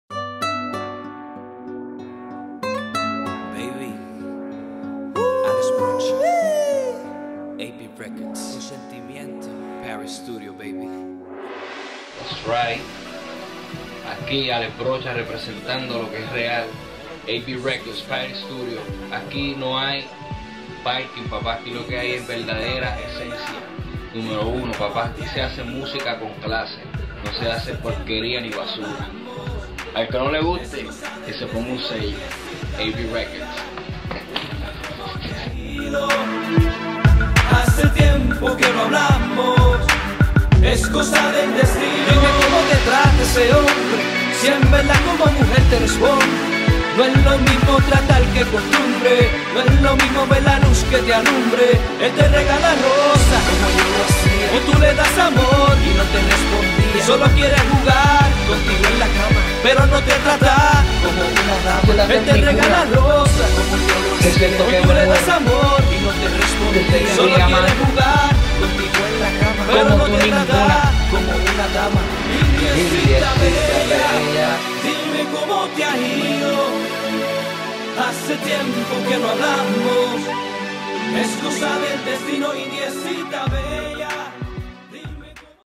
Paris Recording Studio and Alex Brocha
Alex Brocha recording his new cd in Paris recording studio Lancaster, Pa.